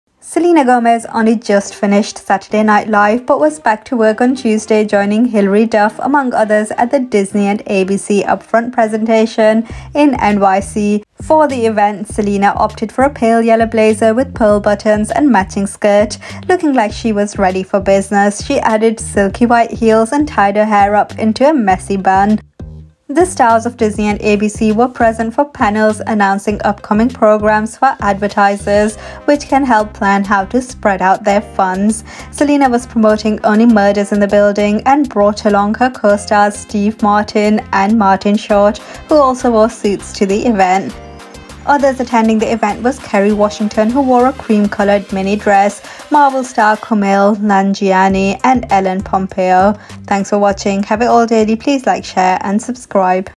Selena Gomez looks ready for business in a miniskirt and blazer at the Disney Upfronts in NYC
Description: Selena Gomez only just finished SNL but was back to work on Tuesday, joining Hilary Duff among others at the Disney and ABC upfront presentation in NYC.

She added silty white heels and tied her hair up into a messy bun.

The stars of Disney and ABC were present for panels anouncing upcoming programming for advertisers which can help plan how to spread out their funds.

Selena was promoting Only Murders in the building and brough along her costars Steve Marting and Martin Short who also wore suits to the event.

Others attending the event was Kerry Washington who wore a cream-colored mini dress, Marvel star Kumail nanjiani and Ellen Pompeo.